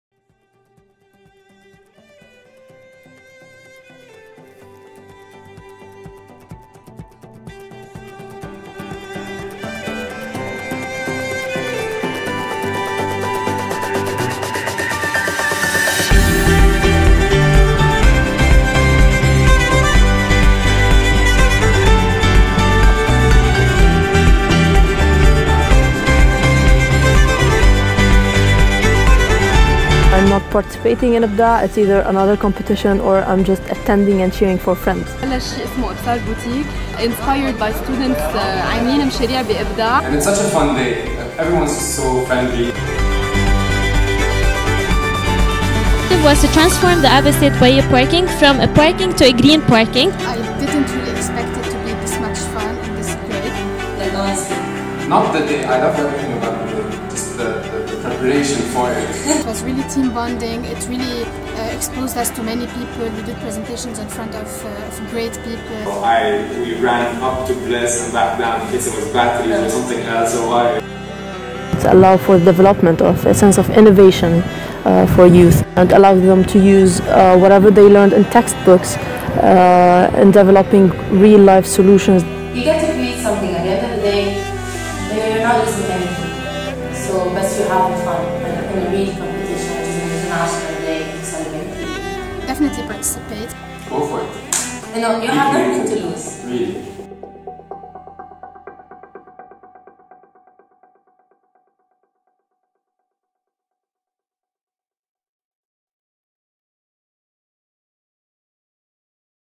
IBDAA compilation - 10th anniversary celebration
IBDAA 2016 is the 10th anniversary of the International Biodiversity Day At AUB! For a decade now, The Nature Conservation Center has invited students to create all kinds of innovative and breathtaking projects that somehow serve nature conservation. To highlight the journey that IBDAA has been on to create this legacy of sustainable thinking, we prepared this short video, featuring some of our favorite moments.

What are yours?